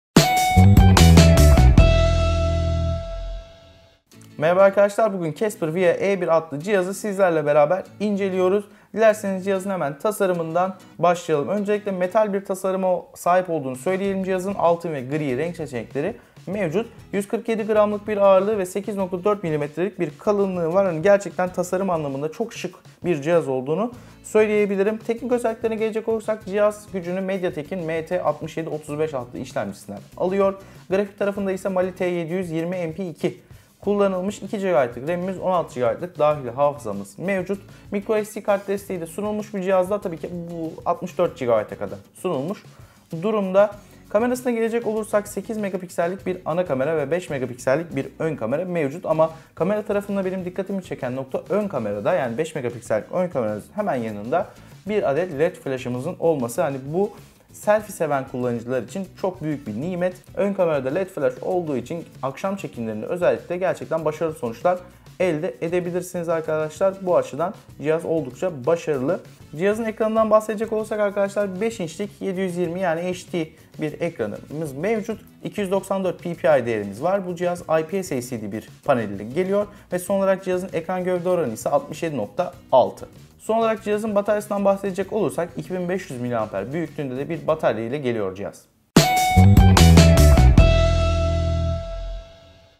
Casper via e1
Casper'ın performansı ve şık tasarımı olan Via E1’i sizler için inceledik!